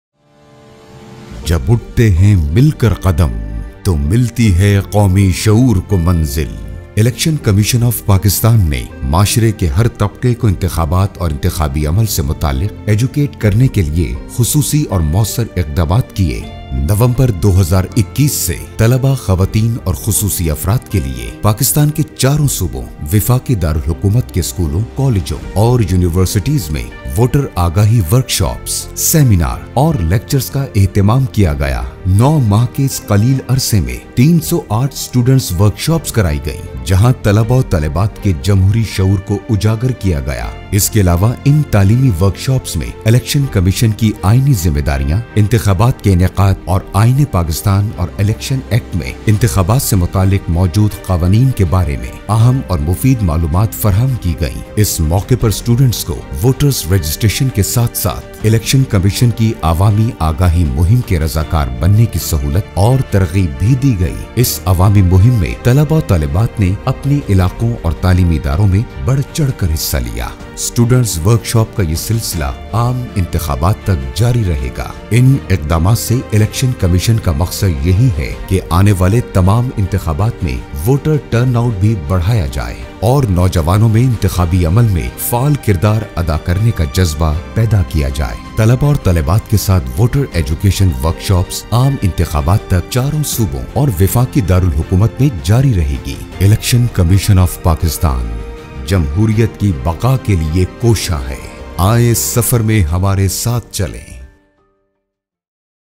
SPECIAL AWARENESS CAMPAIGN TO INCREASE VOTER REGISTRATION AND VOTER TURNOUT
PRESS RELEASE - THE ELECTION COMMISSION IS CONDUCTING A SPECIAL AWARENESS CAMPAIGN TO INCREASE VOTER REGISTRATION AND VOTER TURNOUT FOR MALE AND FEMALE STUDENTS IN VARIOUS EDUCATIONAL INSTITUTIONS ACROSS THE COUNTRY